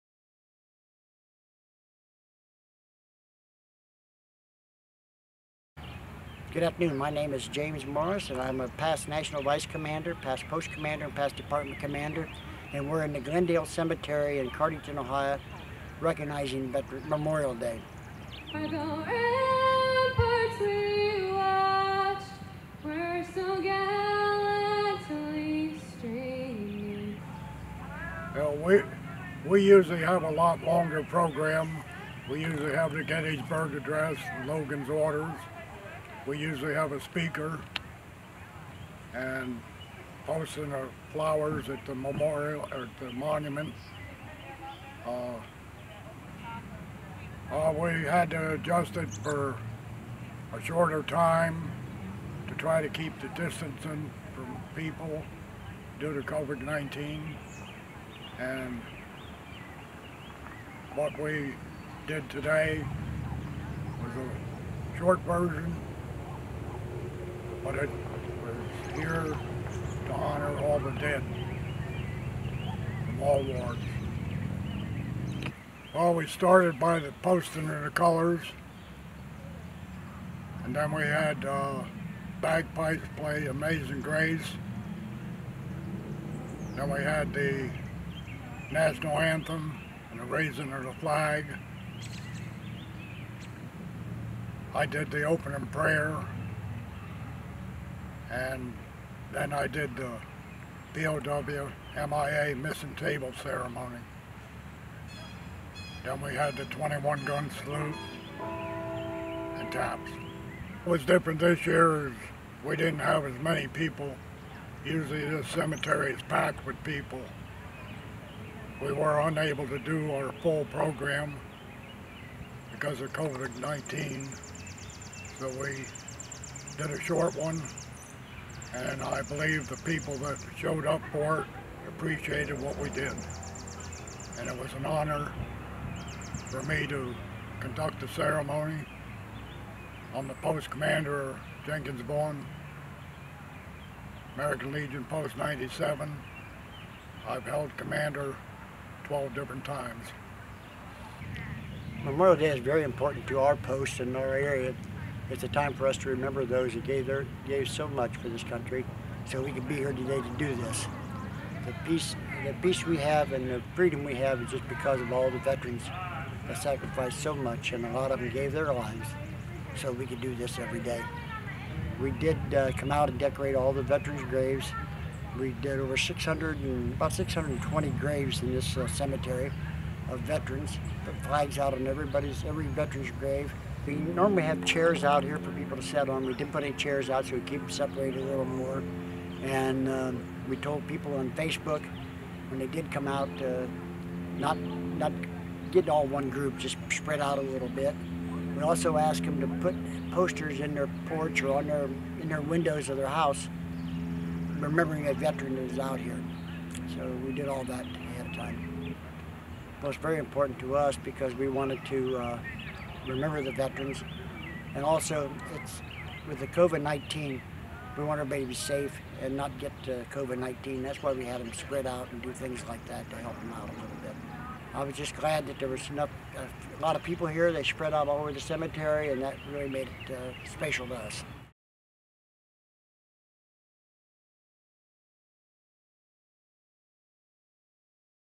Cardington, Ohio American Legion Memorial Day Ceremony
The American Legion Family members from Jenkins-Vaughan Post 97 were forced to cancel their annual Memorial Day parade in Cardington, Ohio due to the COVID-19 pandemic, but, they found a new way to observe Memorial Day with their community.